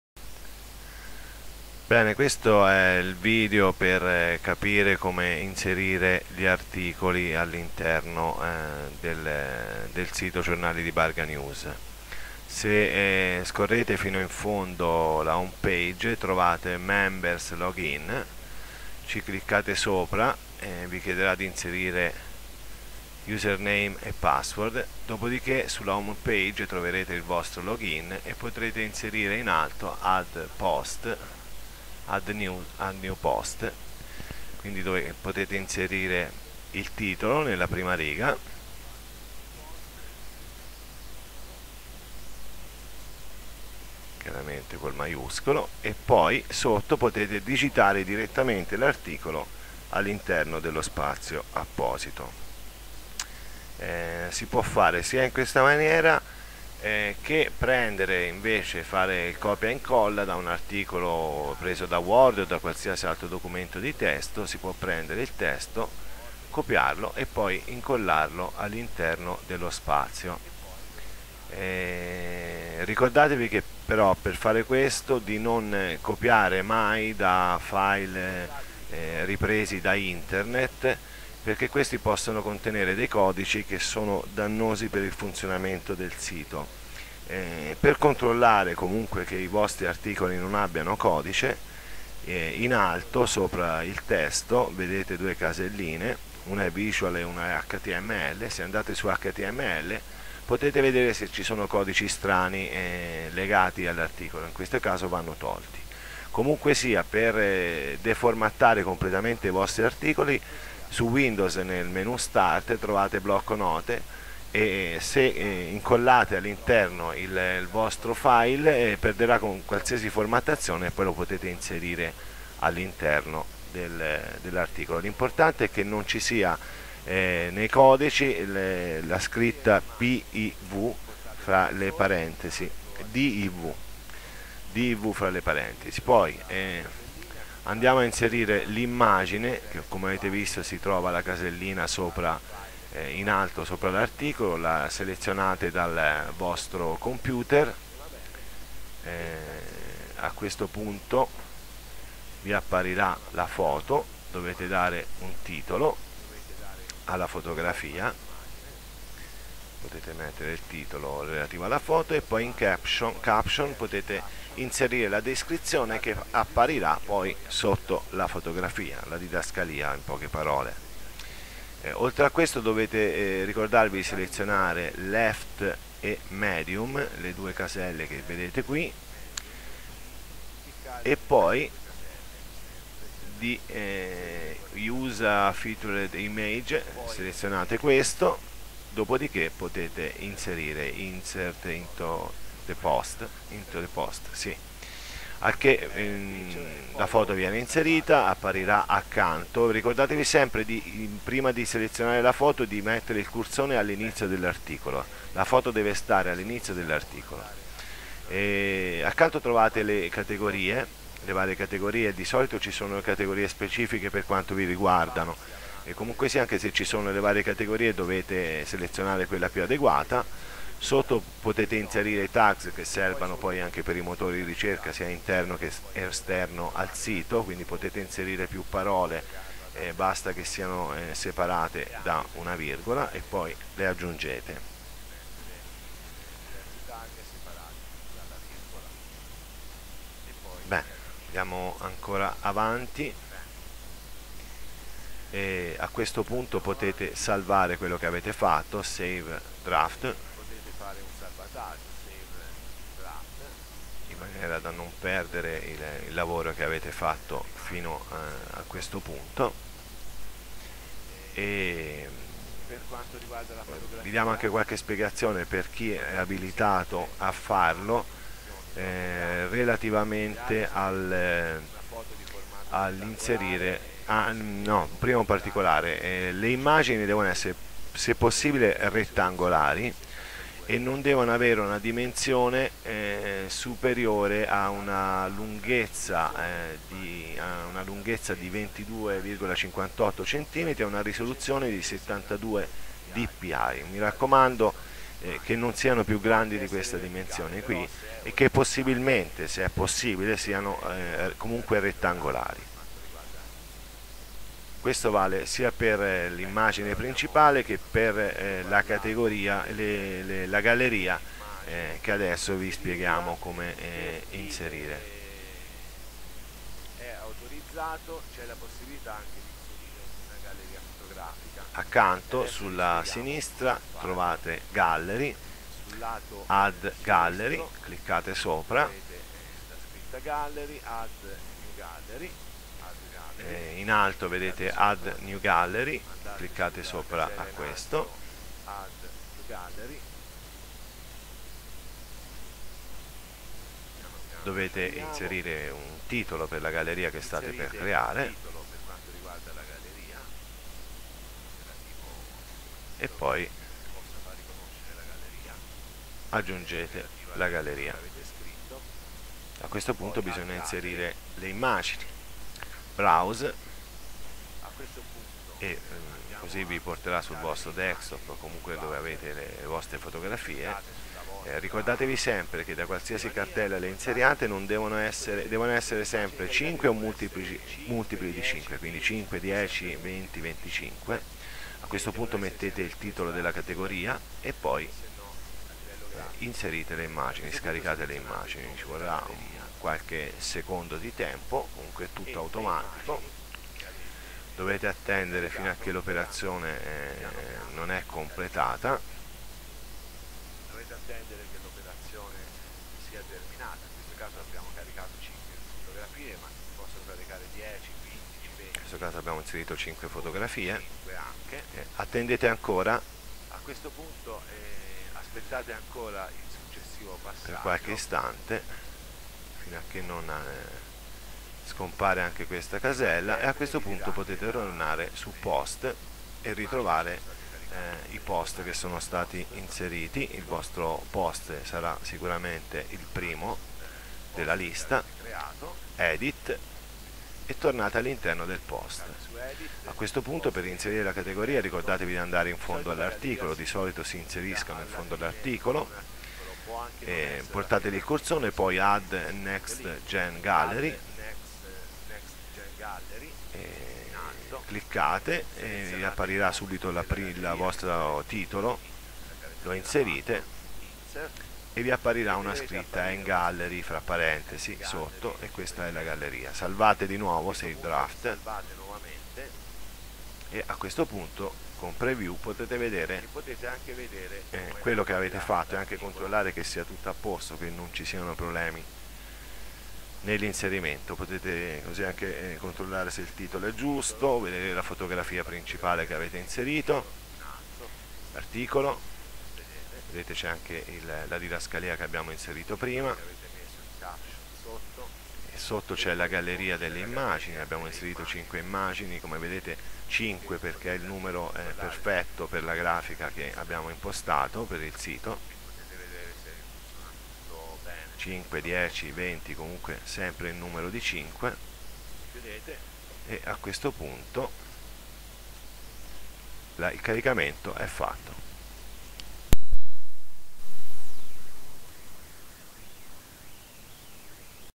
Come inserire articoli e foto
Un video con le istruzioni per pubblicare articoli e foto su
instruction video on posting articles onto (in italian)